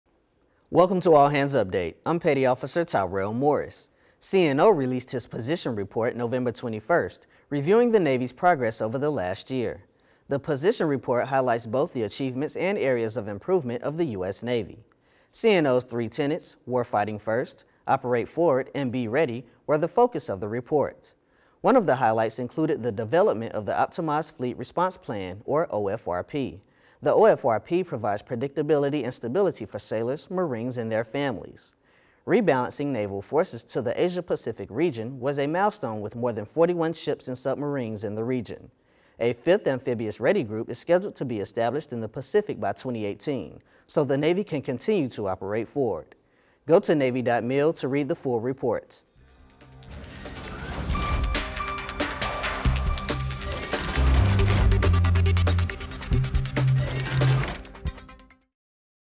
CNO Releases His 2014 Position Report
All Hands Update November 24, 2014 #2
CNO released his position report November 21st reviewing the Navy's progress over the last year.